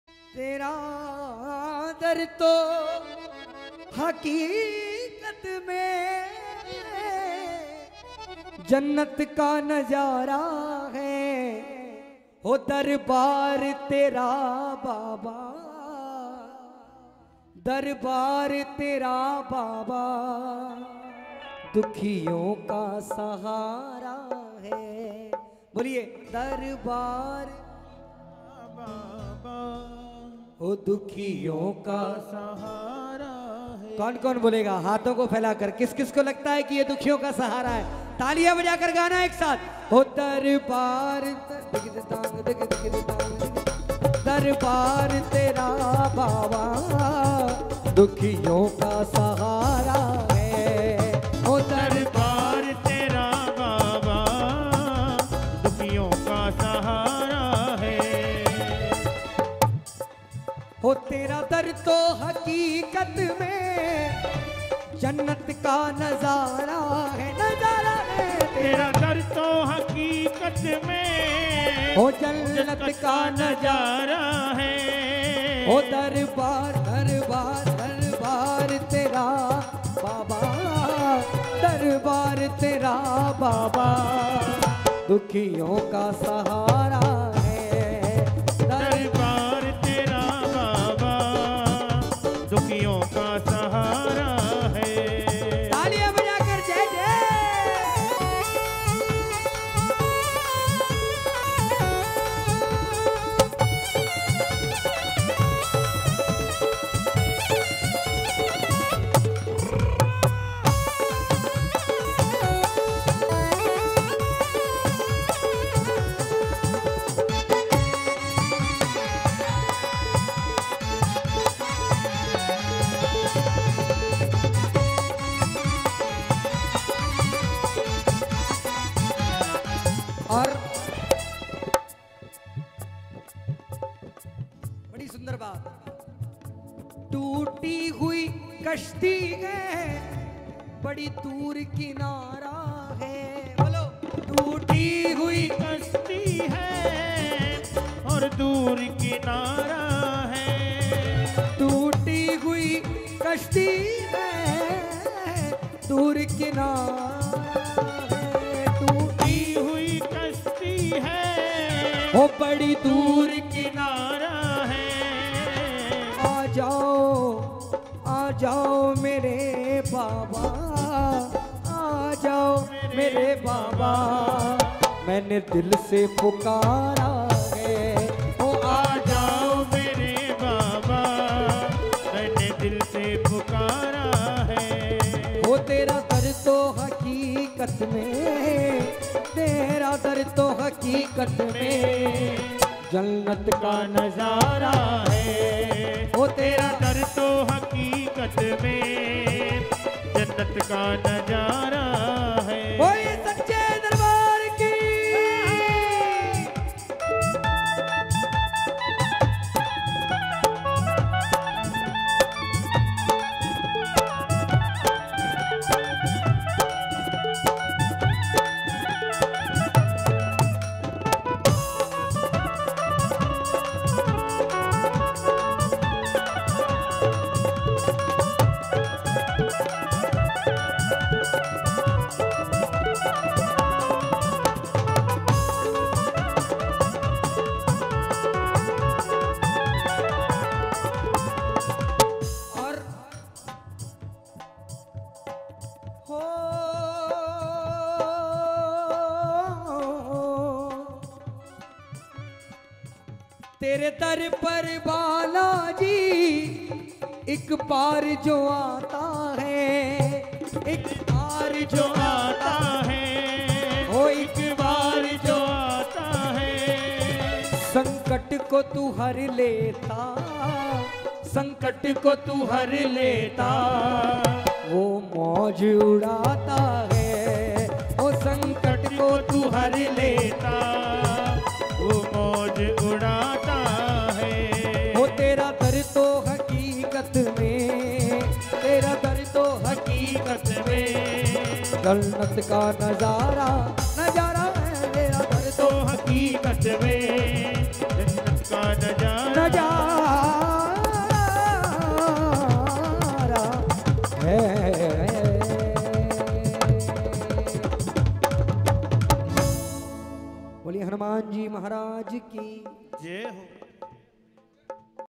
Ajay Dousa -48वा श्री बालाजी महाराज जन्मोत्सव-15.4.2022 ( श्री बालाजी सत्संग समिति ) Sumirau Shyam
!! Sumirau shyam kanpur !!  Live Telecast From Khatu Dham